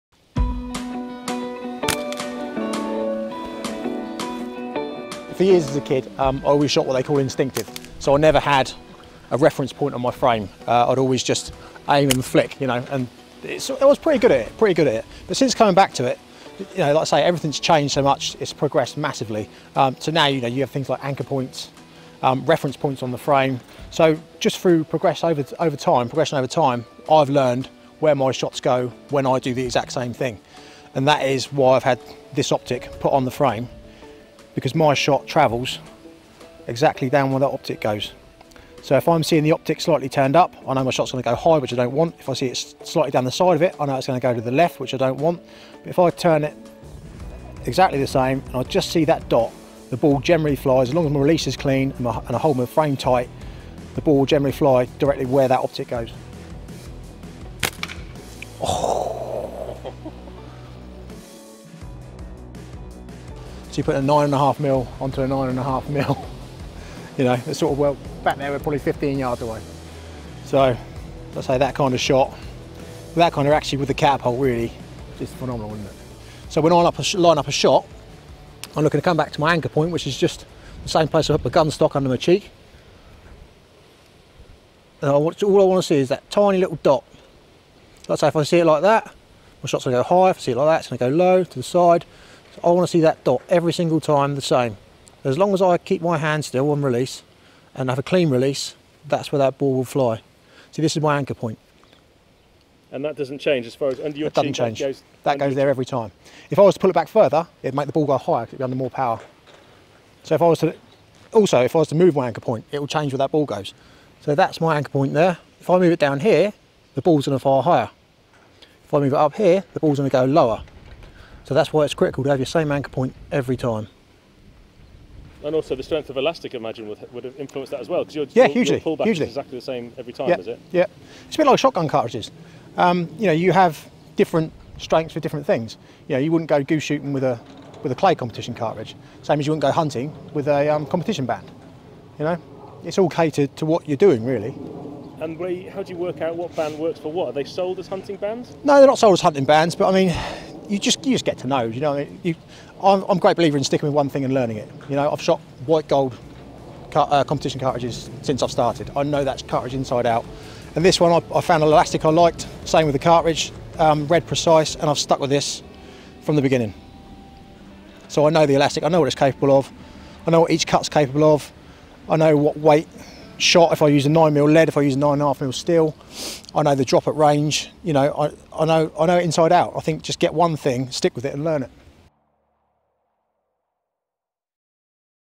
Catapult sights and anchor points
Catty Shack man Wayne Martin shows how to improve your slingshot shooting accuracy, using reference points on your body, on your catapult - and even optical sights.

▶ Join the Fieldsports Nation. Just £/$/€4.99 a month gets you Fieldsports Channel membership on YouTube.